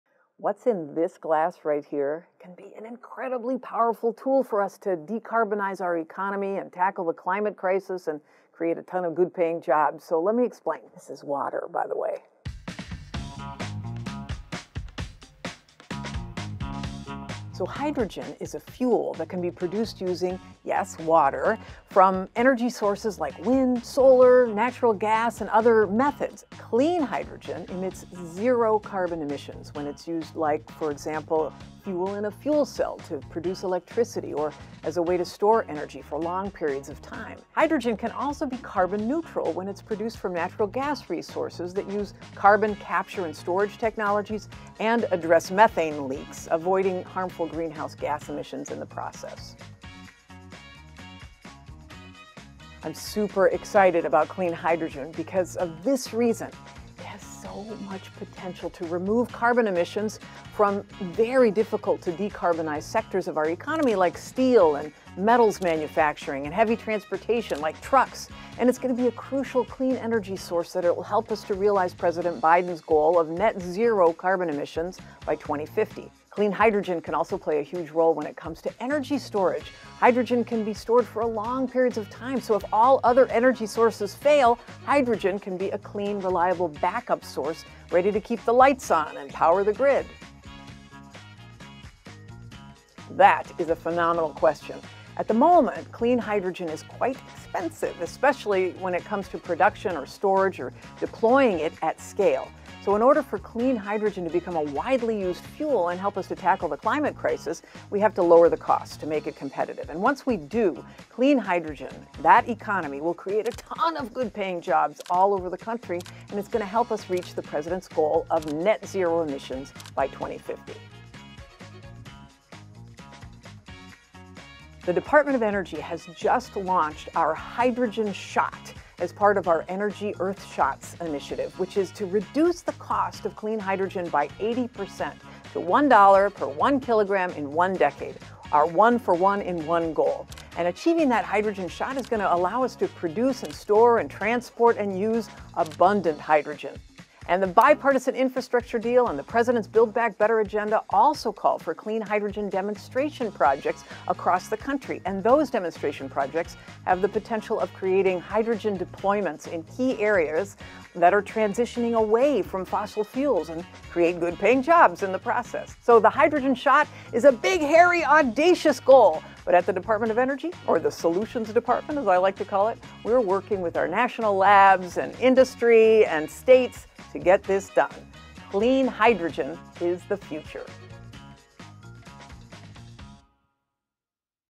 Secretary Jennifer Granholm Explains Clean Hydrogen
Clean energy sources like, hydrogen, will help us combat climate change by addressing industrial emissions that are often hard to decarbonize. And, it can be produced from a variety of clean energy sources including renewables and nuclear.  Watch U.S. Energy Secretary Jennifer Granholm explain on how the Department of Energy is making this a reality.

Learn more about our "Hydrogen Shot" initiative to reduce the cost of hydrogen by 80% — a program that will help us decarbonize our economy and create good-paying union jobs across America, including in areas that are transitioning away from fossil fuels